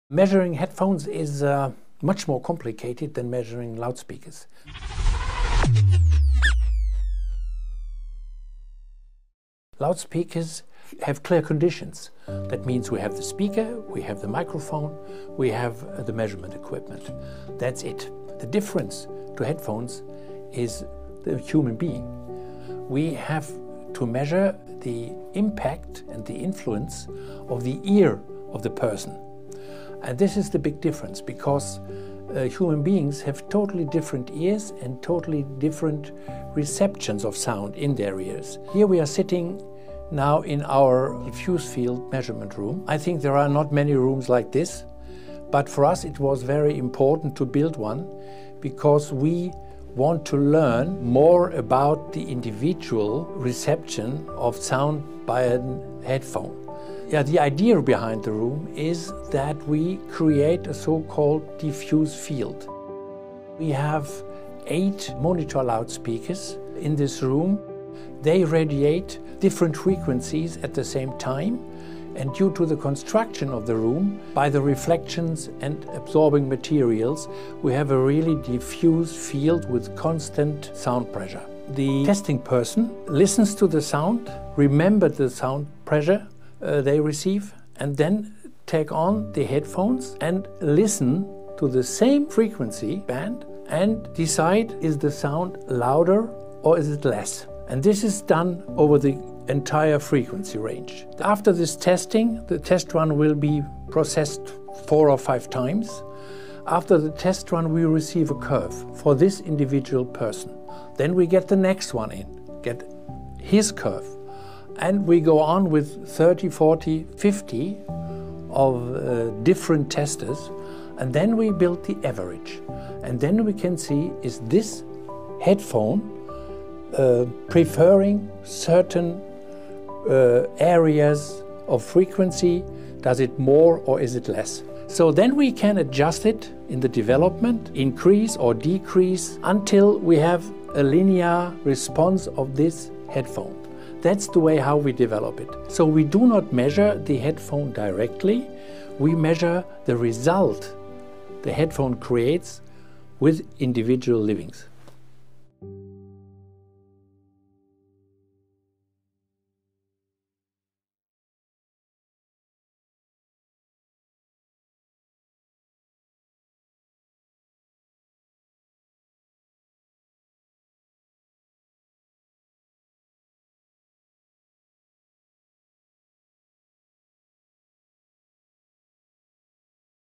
Harman-Curve Competitor? Measuring Headphones Using Humans at Germany's T+A (August 2023)
T+A is an innovative German brand obsessed with product quality and the science of sound. So when T+A first entered the headphone market with its Solitaire P headphones, which were followed by the Solitaire P-SE and T models, the company wanted to offer consumers something different in terms of design and sound.

In this video, founder Siegfriend “Siggi” Amft describes how T+A arrives at the unique sound signature for its headphones—by not measuring the headphones directly, but by using dozens of real listeners who evaluate the sound of the headphones under development against the sound of speakers in a controlled listening room.